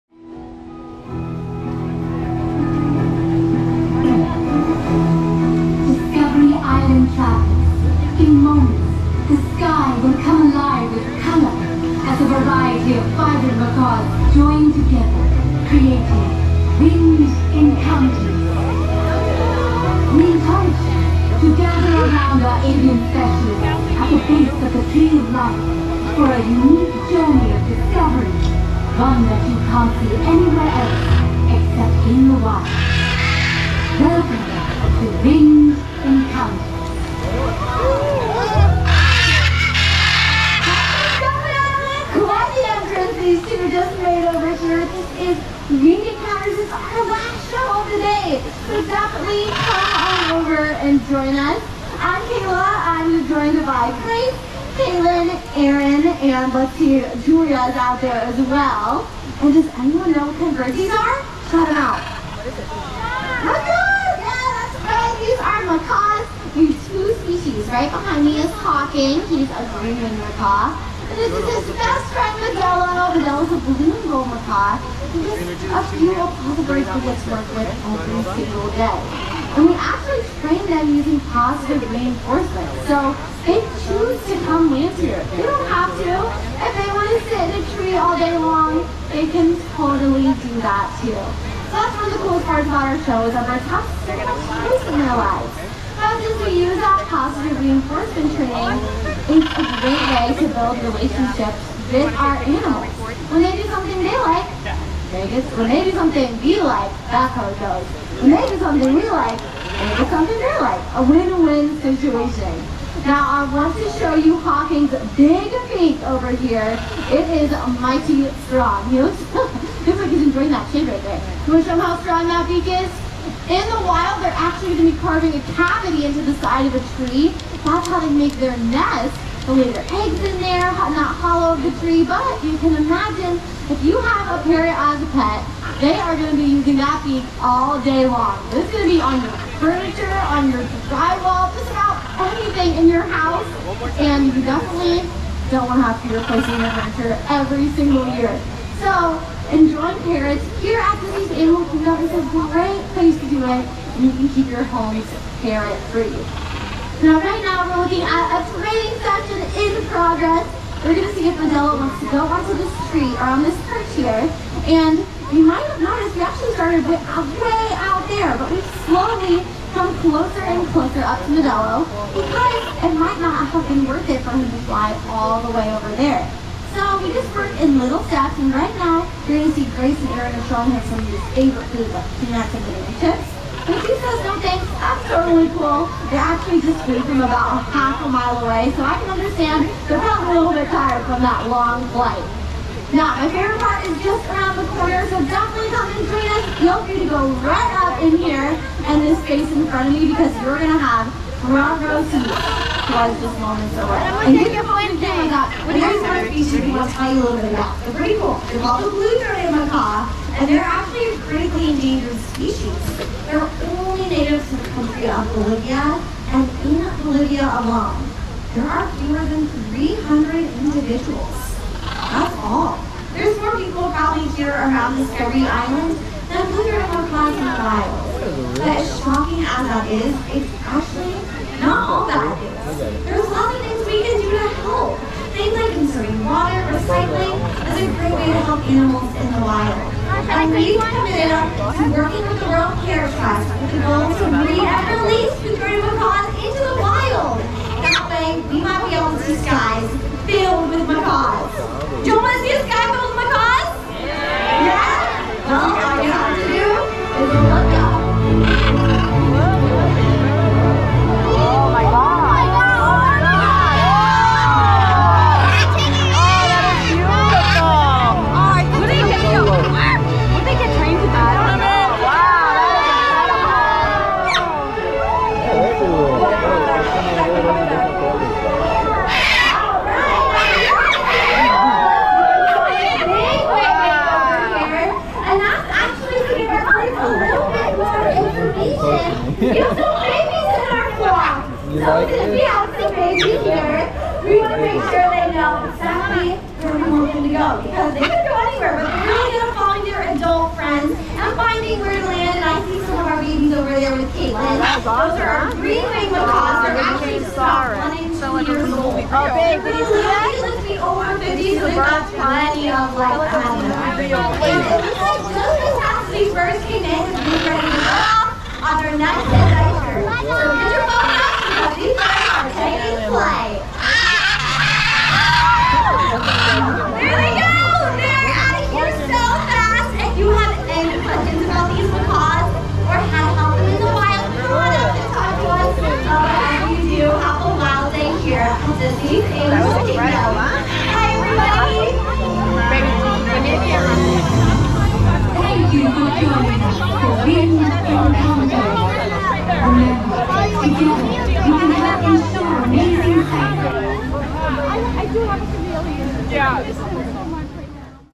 Winged Encounters - The Kingdom Takes Flight | Disney's Animal Kingdom | FULL SHOW 4K Ultra HD
Animal Encounters: Winged Encounters – The Kingdom Takes Flight is a show at Disney's Animal Kingdom, where you will have an ultimate bird-watching experience as the fascinating birds take off near the Tree of Life.

Turn your eyes to the skies to watch a free-flying flock of magnificent macaws as the brilliantly colored birds swoop and soar over Discovery Island. You will encounter 6 species of South American macaw: hyacinth, green-winged, blue and gold, scarlet, blue-throated and military. The largest have an incredible wingspan of around 48 inches.

👉GoPro HERO10 Black
👉GoPro Shorty Mini Extension Pole Tripod